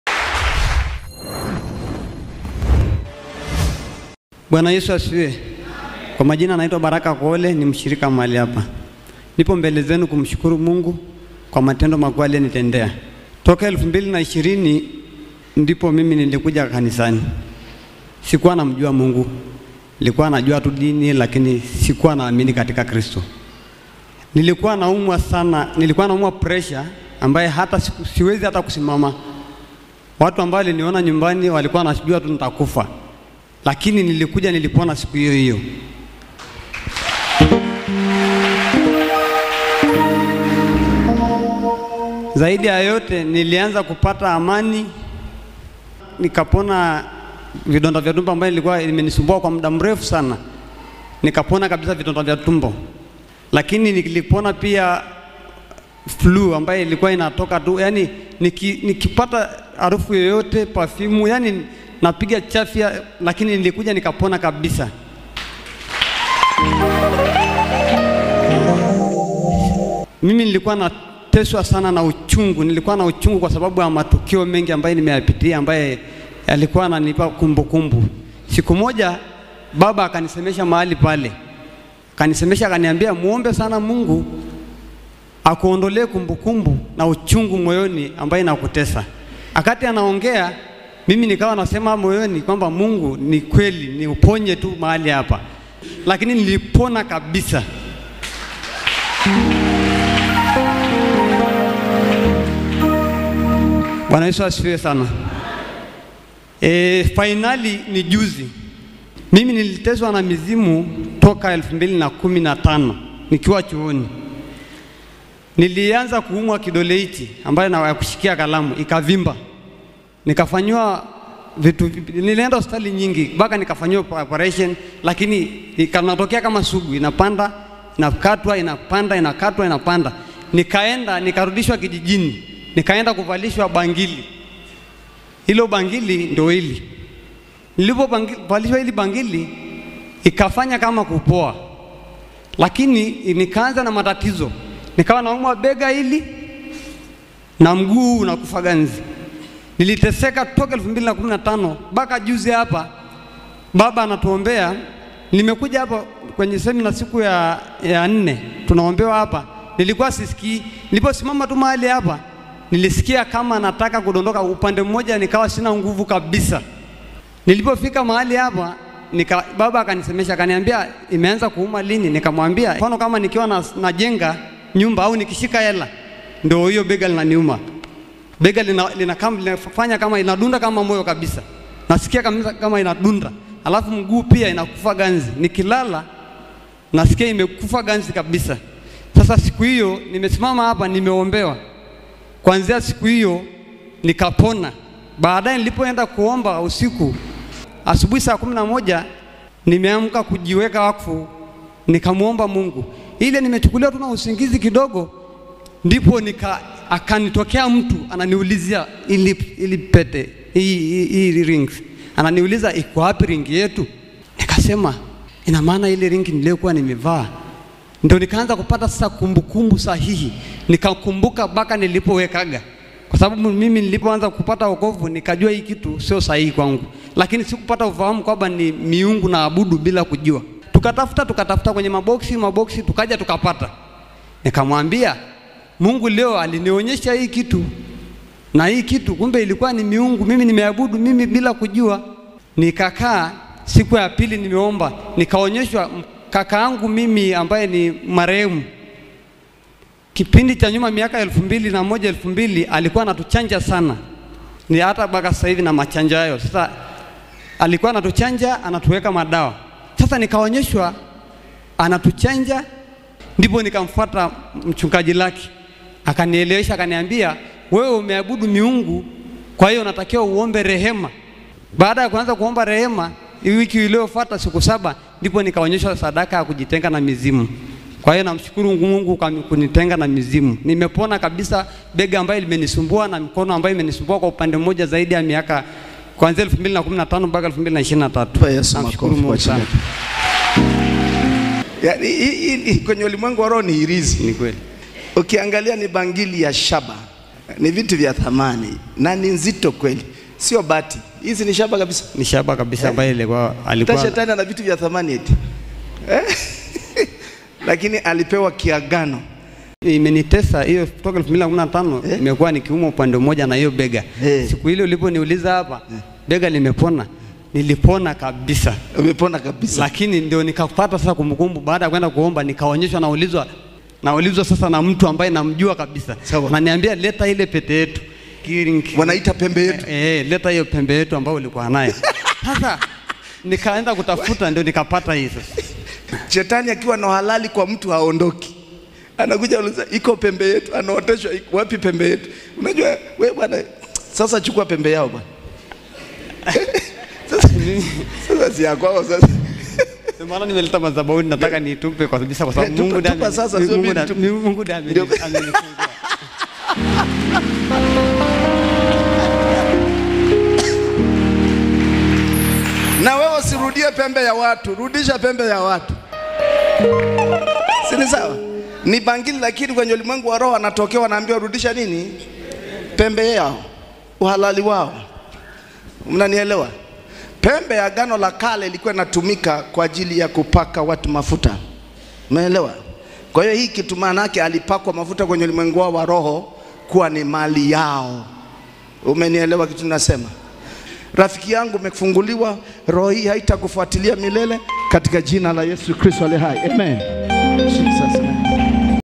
SIKUJUA KAMA HII BANGILI INA MAAGANO YA KUZIMU
Bishop Elibariki Sumbe Kiongozi wa Huduma ya Vuka Yordani na Mwangalizi na Wa Makanisa Ya Grace Evangelical Church Tanzania (G.E.C.T)

UBARIKIWE.